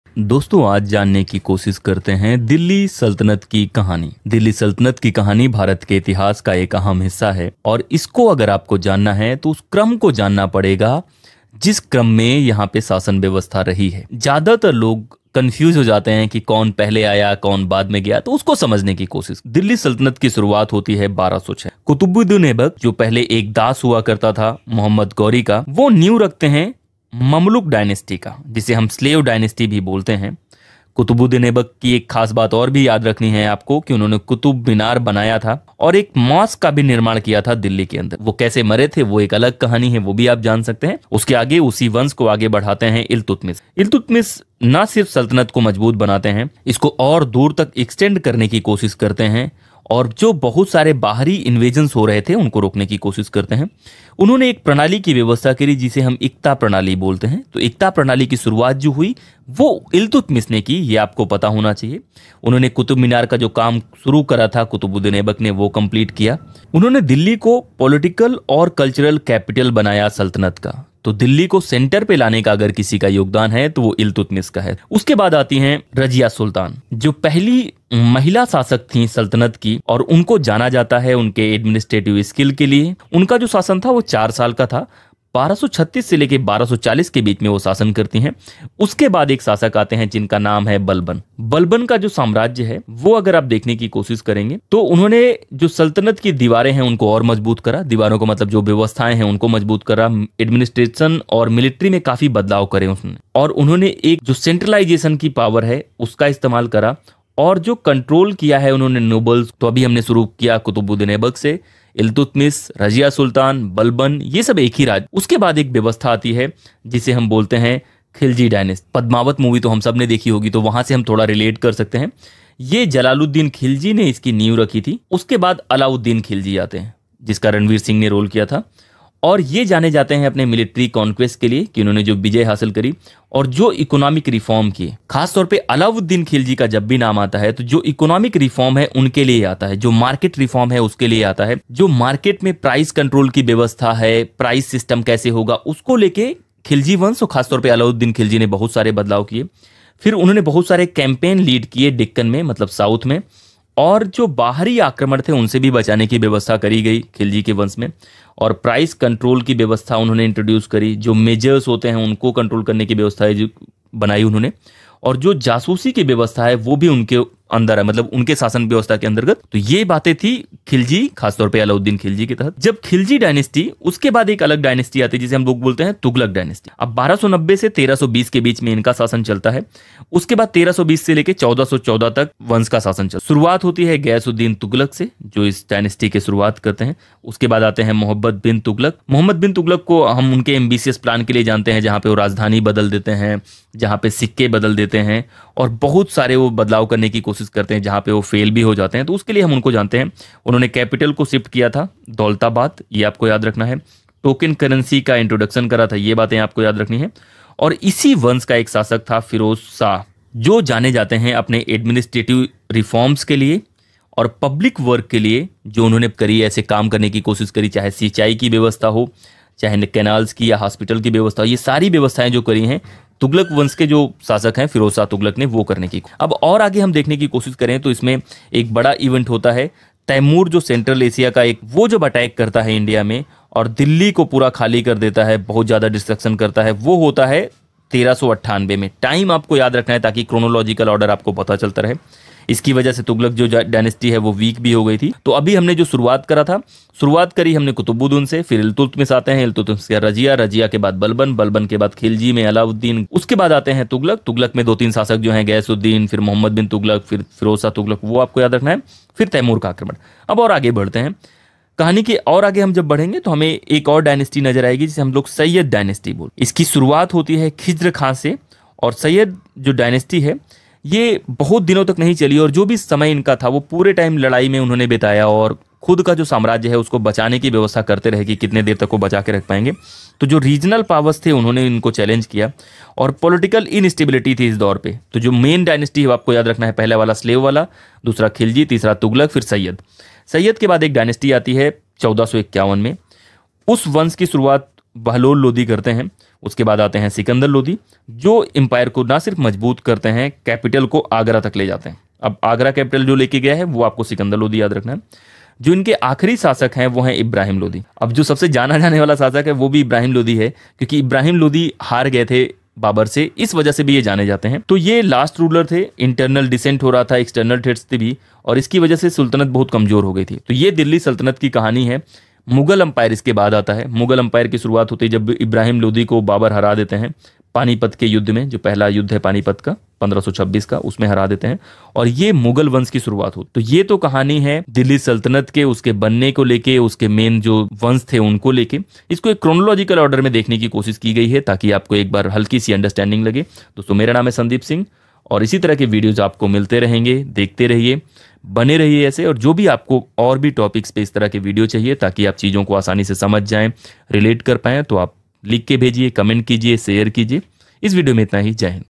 The Story of the Delhi Sultanate | Slave Dynasty to Lodhi Dynasty
Exploring the Story of the Delhi Sultanate

Friends, today we will delve into the history of the Delhi Sultanate, a significant part of India's past. To understand it fully, we need to follow the chronological order of the ruling dynasties. Many people get confused about who came first and who followed. Let's clear that up.

Video Highlights:

Foundation of the Delhi Sultanate:

Qutb-ud-din Aibak: The Delhi Sultanate begins in 1206 with Qutb-ud-din Aibak, a former slave of Muhammad Ghori, who established the Mamluk (Slave) Dynasty.
Contributions: He built the Qutub Minar and a mosque in Delhi. Remember, he started the construction which was completed later.

Successors of Aibak:

Iltutmish: Strengthened and expanded the Sultanate. Introduced the Iqta system and completed the Qutub Minar. Made Delhi the political and cultural capital.
Razia Sultan: The first female ruler known for her administrative skills. Ruled from 1236 to 1240.
Balban's Rule:Ghiyas ud din Balban: Reinforced the administration and military, centralized power, and controlled the nobles.

Khilji Dynasty:

Jalal-ud-din Khilji: Founder of the Khilji Dynasty.
Alauddin Khilji: Known for his military conquests and economic reforms, particularly in price control and market regulations. Also led campaigns in the Deccan and protected the empire from external invasions.

Tughlaq Dynasty:

Ghiyas-ud-din Tughlaq: Founder of the Tughlaq Dynasty.
Muhammad bin Tughlaq: Known for his ambitious plans like shifting the capital to Daulatabad and introducing token currency, which largely failed.
Firoz Shah Tughlaq: Recognized for administrative reforms and public works like irrigation canals and hospitals.

Significant Events:

Timur's Invasion (1398): Caused massive destruction in Delhi, weakening the Tughlaq Dynasty.

Sayyid Dynasty:

Khizr Khan: Founder of the Sayyid Dynasty, which struggled to maintain control and faced constant challenges from regional powers.

Lodi Dynasty:

Bahlul Lodi: Established the Lodi Dynasty in 1451.
Sikandar Lodi: Strengthened the empire and moved the capital to Agra.
Ibrahim Lodi: The last ruler defeated by Babur in the Battle of Panipat (1526), leading to the beginning of the Mughal Empire.

About the Channel:
This video provides a comprehensive look at the Delhi Sultanate, from its foundation to the major dynasties that ruled it. My name is Sandeep Singh, and I aim to make complex historical narratives accessible and engaging.